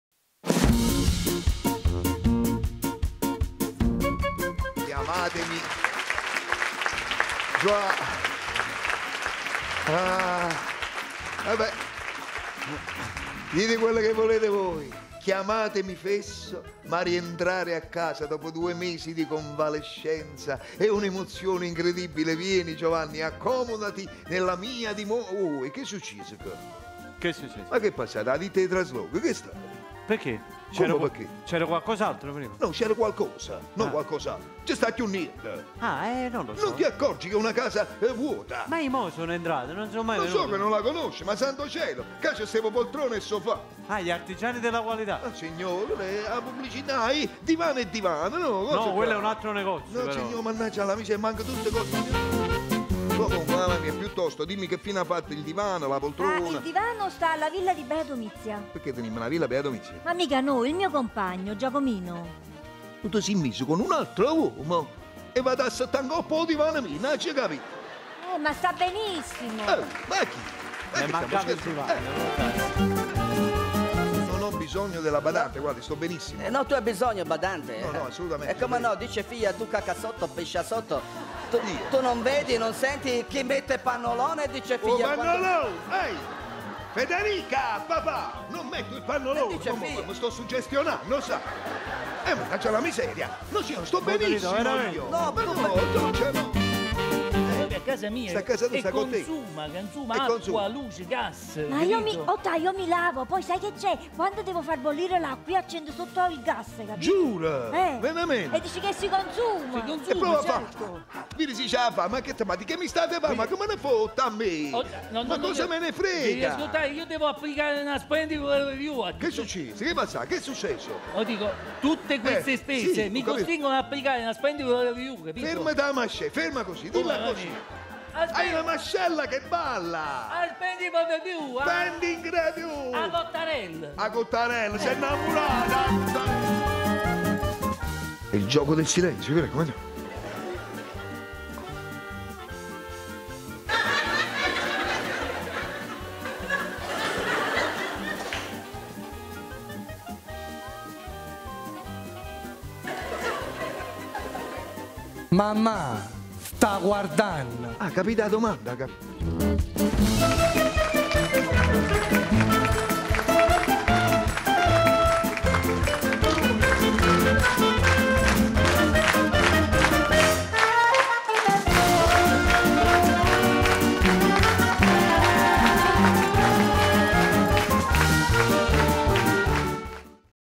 "Con tutto il cuore": il promo della commedia di Vincenzo Salemme
In scena al Teatro Manzoni di Milano dall'11 dicembre 2018 al 1° gennaio 2019. Scritto, diretto e interpretato dal regista napoletano, lo spettacolo vede protagonisti anche Domenico Aria, Vincenzo Borrino, Antonella Cioli, Sergio D'Auria, Teresa Del Vecchio, Antonio Guerriero, Giovanni Ribò e Mirea Flavia Stellato.